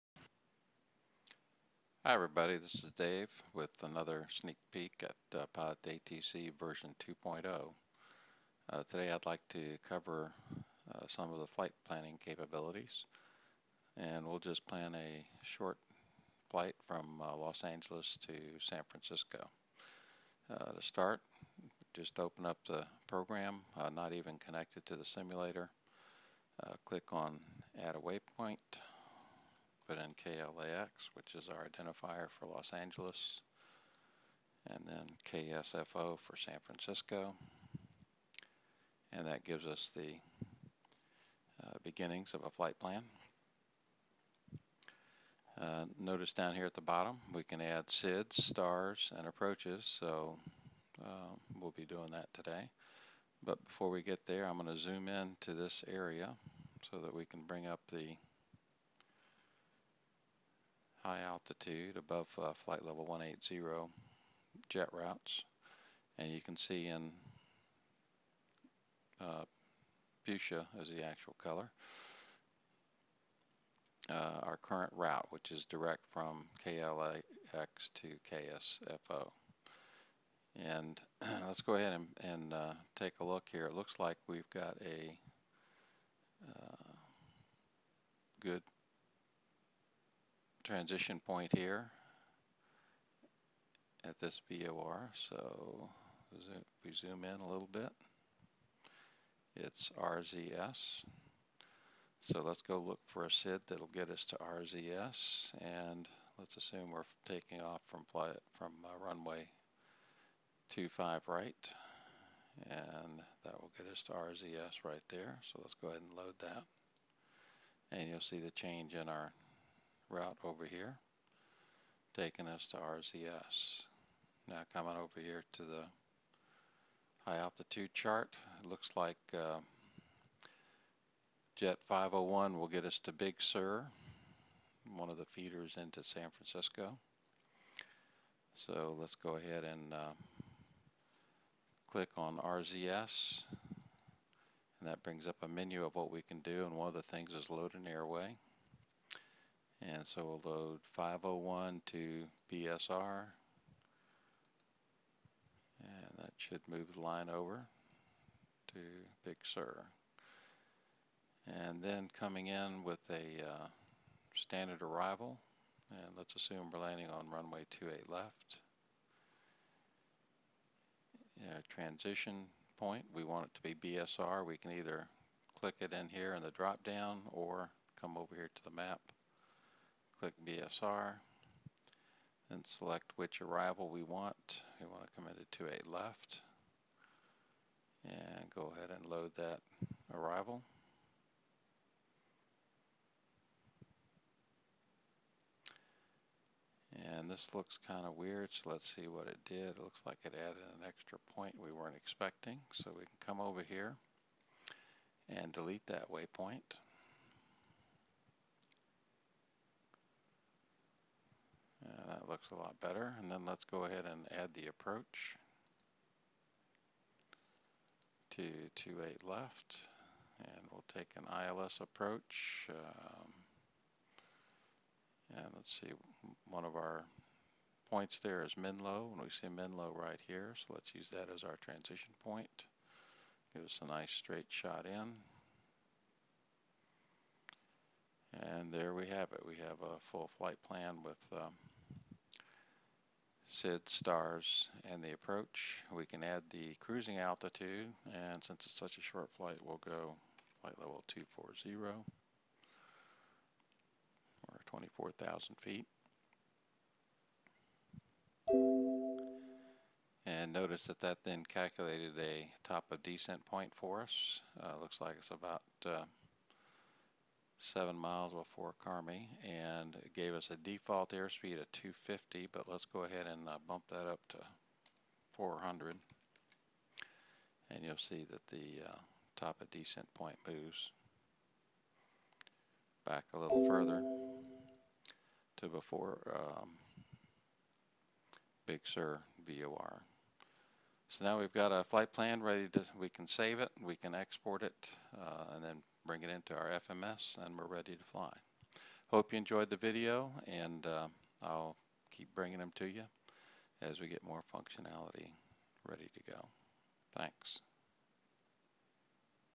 Pilot2ATC v2.0 IFR FlightPlan Sneak Peak
Short video of a quick IFR flight plan creation from KLAX to KSFO with SID, STAR and ILS Approach.  Just a peak at some of the new functionality in v2.0.